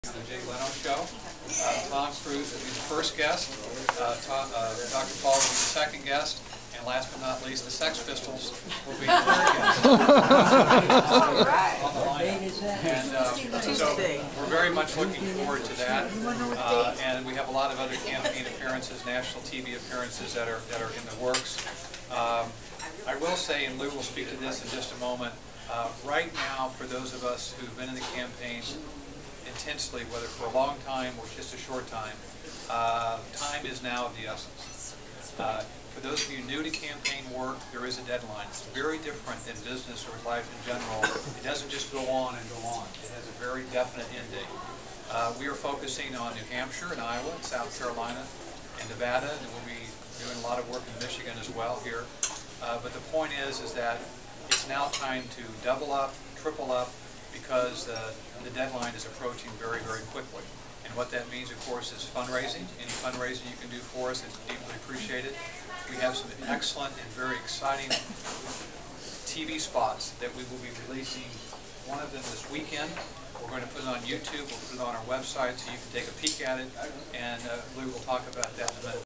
Ron Paul Campaign Manager - Jay Leno Announcement
The Ron Paul campaign manager Kent Snyder talks about Ron Paul's upcoming schedule after dinner at La Shish Restaurant.

Ron Paul will be on the Tonite Show with Jay Leno on Tuesday October 30.